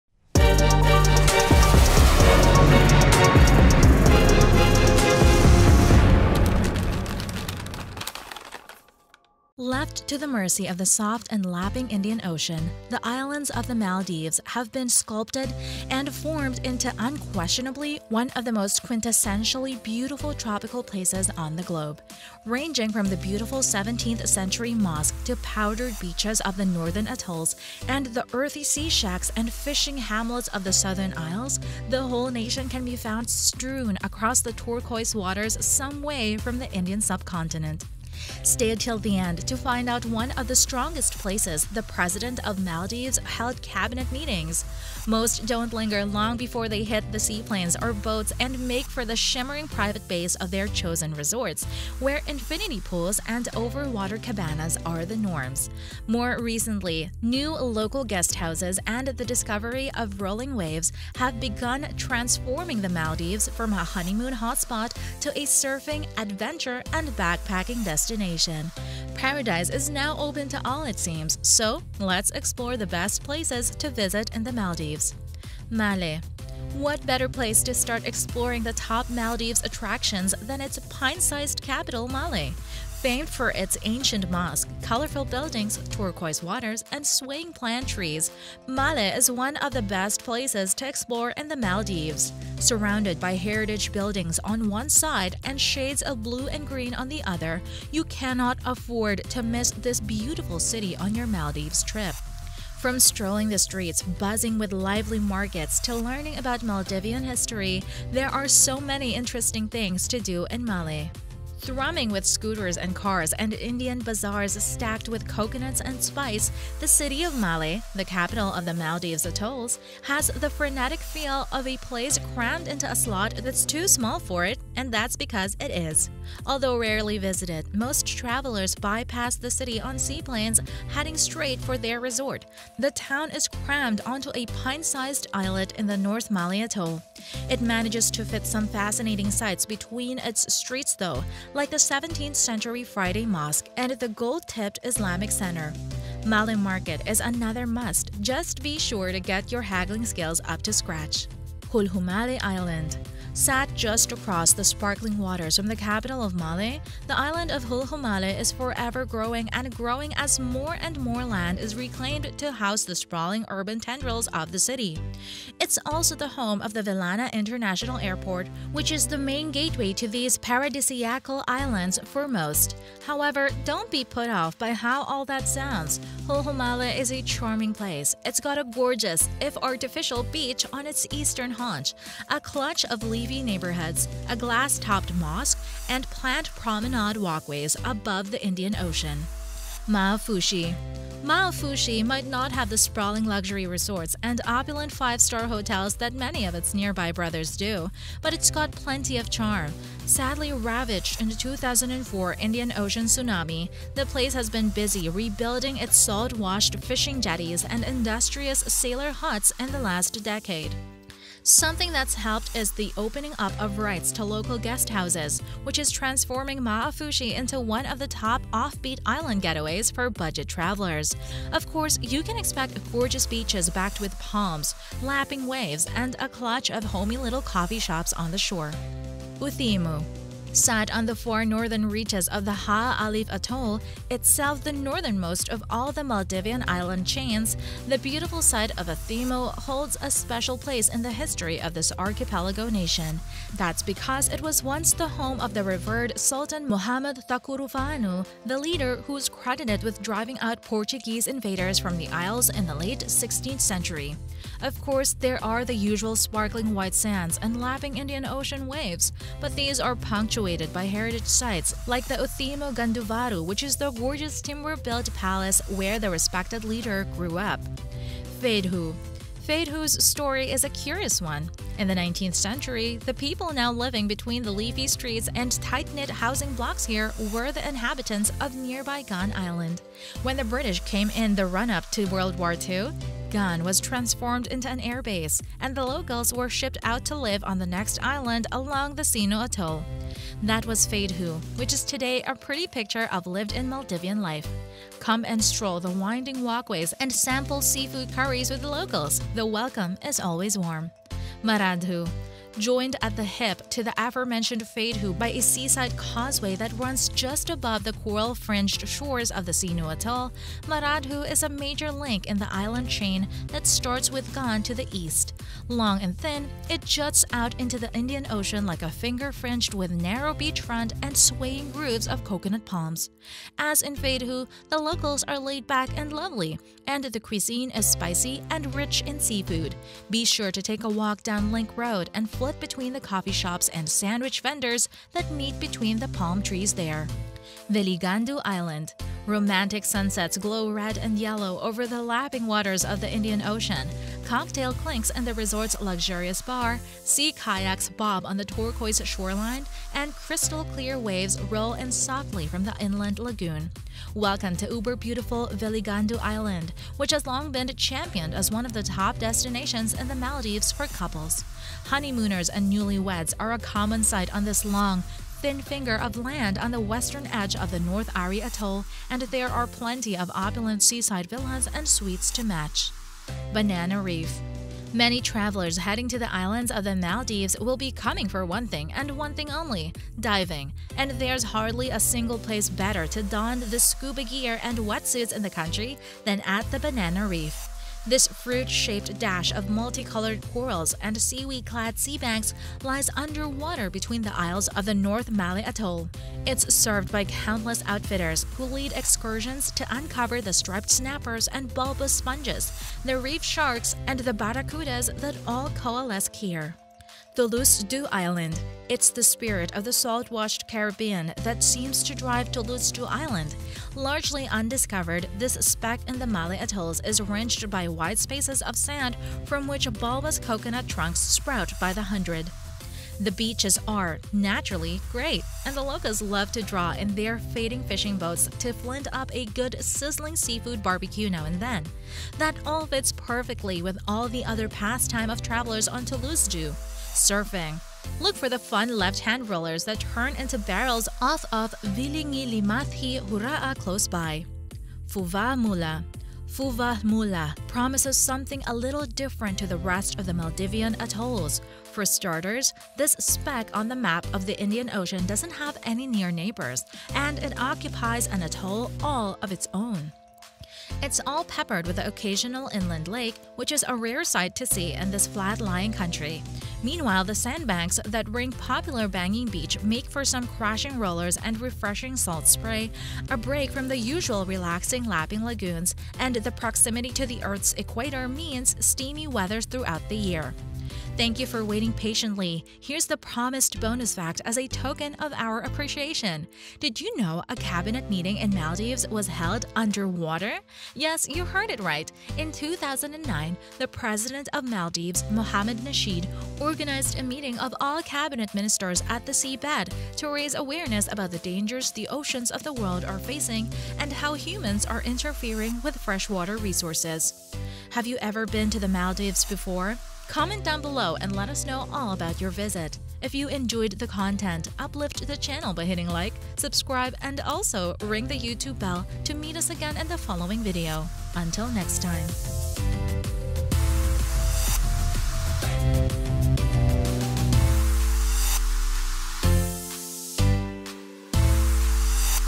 Maldives Travel Guide: The Best Maldivian Places (Travel Video)
Mladives is tourists' absolute favorite when it comes to visiting countries in South Asia. Maldivian places like Male, Hulhumale Island, and Maafushi are a few of the many destinations you will be visiting in this travel guide.

0:00 Maldivian Best Places to Visit
1:15 Male
2:31 Hulhumale Island
3:09 Maafushi
3:49 Utheemu
4:28 Feydhoo
5:01 Maradhoo
5:37 Veligandu Island
6:13 Banana Reef
6:47 Thulusdhoo Island
7:25 Fuvahmulah